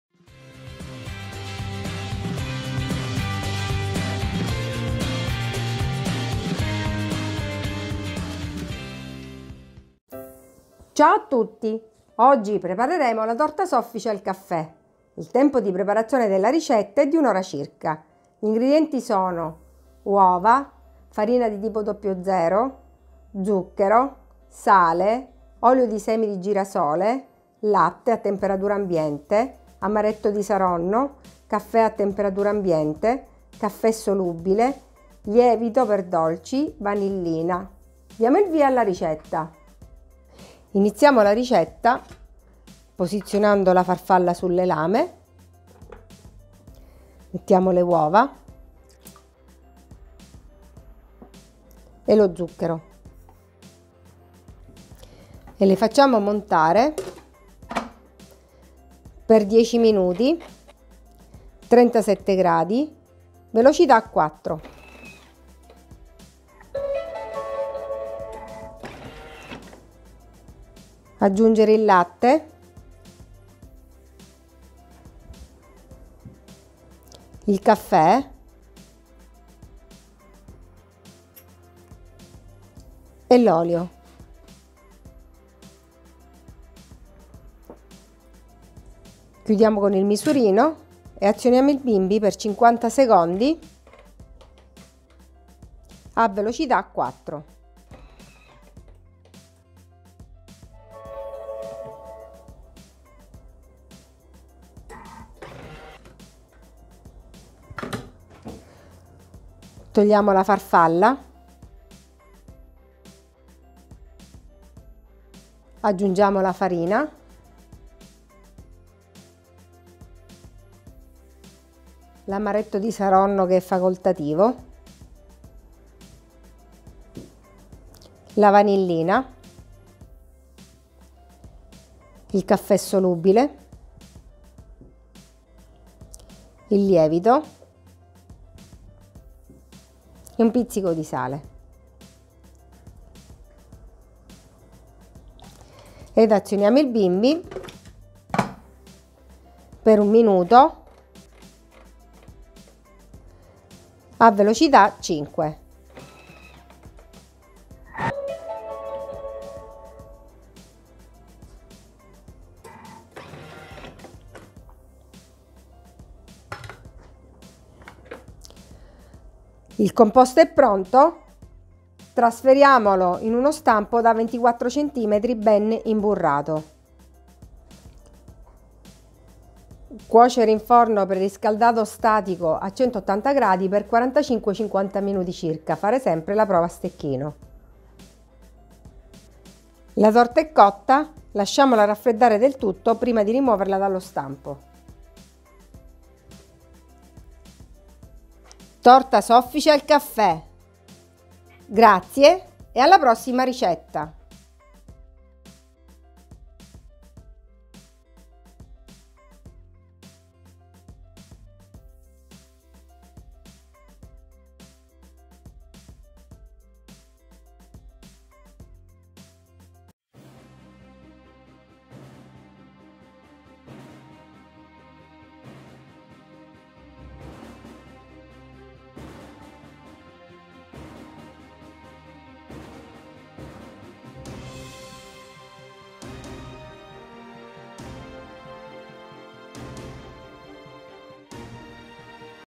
Torta soffice al caffè per bimby TM6 TM5 TM31
Ingredienti:
3 uova
250 g di farina di tipo "00"
180 g di zucchero semolato
1 pizzico di sale
120 g di olio di semi di girasole
60 g di latte a temperatura ambiente
30 g di amaretto di Saronno (facoltativo)
60 g di caffè espresso a temperatura ambiente
2 cucchiaini di caffè solubile
1 bustina di lievito per dolci vanigliato
1 bustina di vanillina